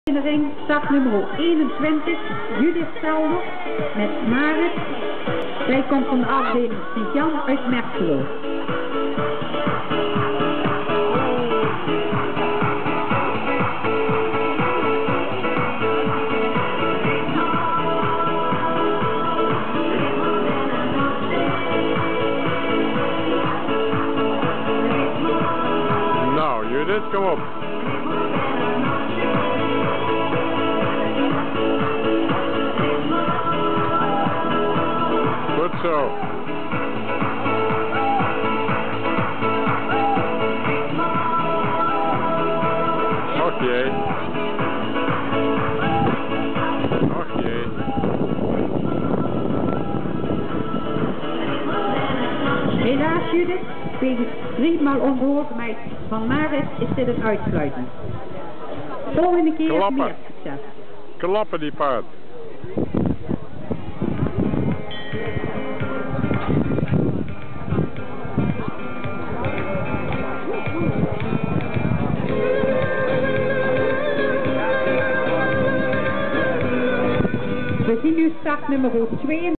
Amazone Judith tijdens haar tweede proef
Het concours "Outdoor 2010 " van de rijvereniging st.Jan te Merselo (L)